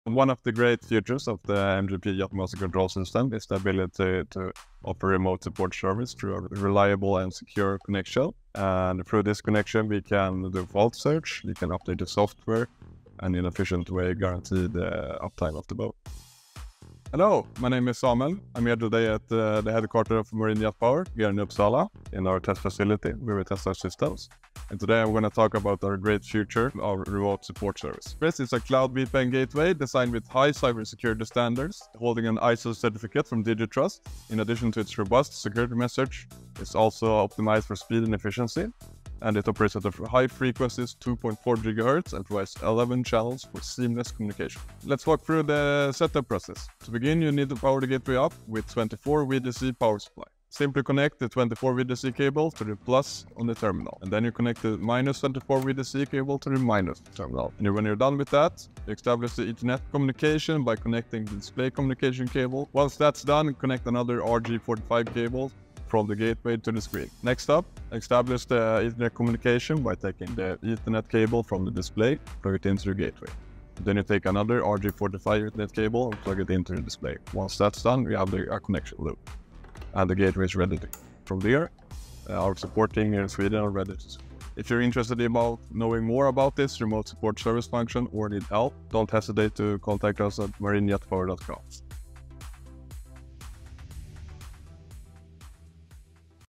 How To Install A Remote Gateway For MJP Waterjet Controls
In this video, we’ll hear from Samuel on how to use a secure internet signal to troubleshoot system faults and download system upgrades.

Caption Correction: The Ethernet cable connected is an "RJ45"